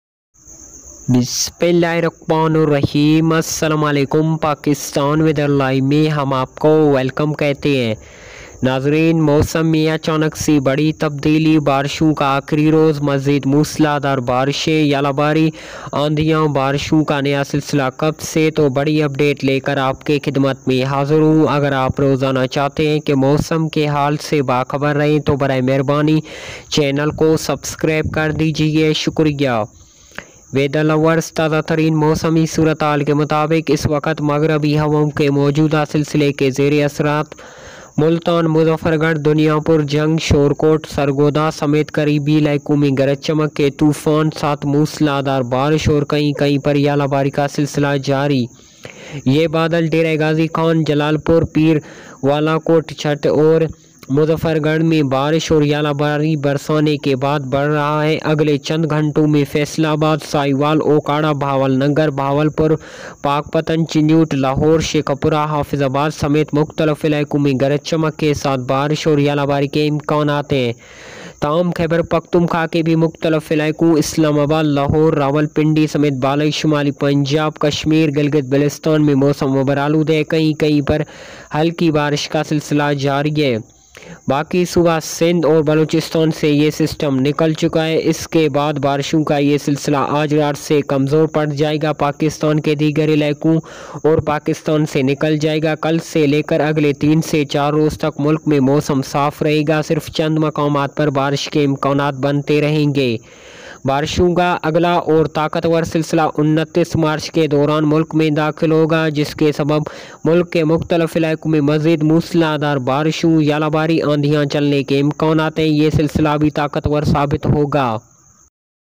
Pakistan Weather Report | Heavy thunderstorms again reached | Stormy rains hails arrived in Pakistan
Viewers.
Sudden change in weather. More heavy rains on last day of rains. Snow. The storms Since the new series of rains, I am at your service with a big update.